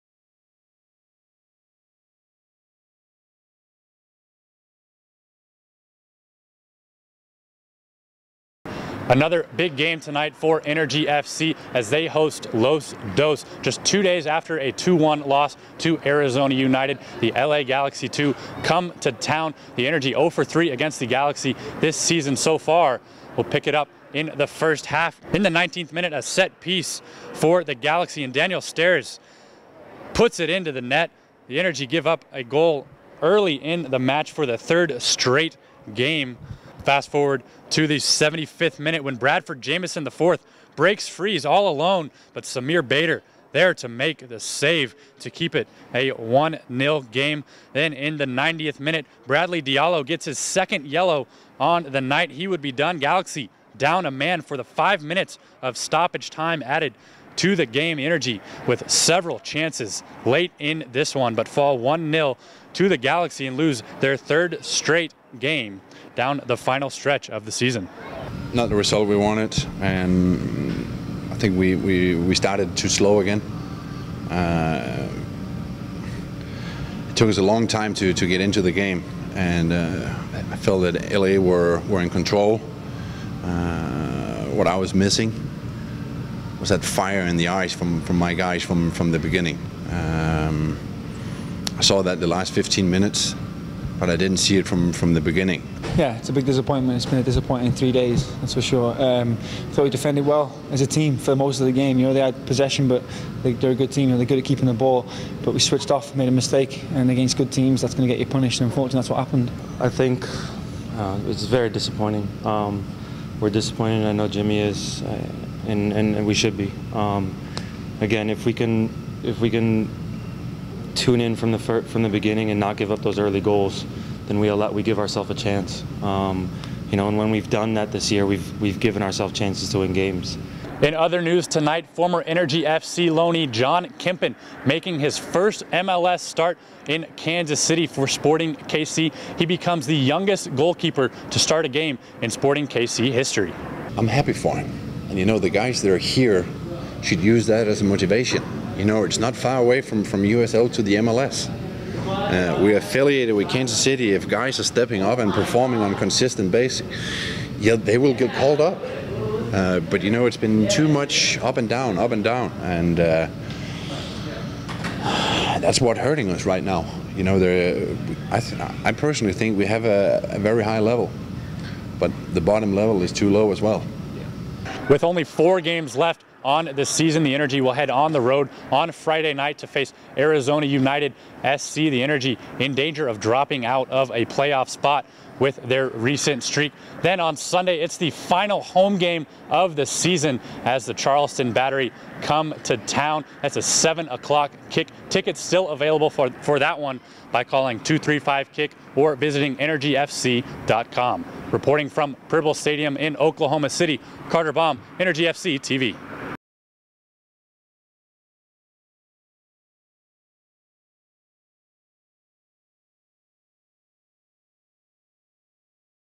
Energy FC Drop Big Game to Galaxy II, 1-0
Energy FC TV's Carter Baum recaps Energy FC's 1-0 loss to LA Galaxy II on Saturday night.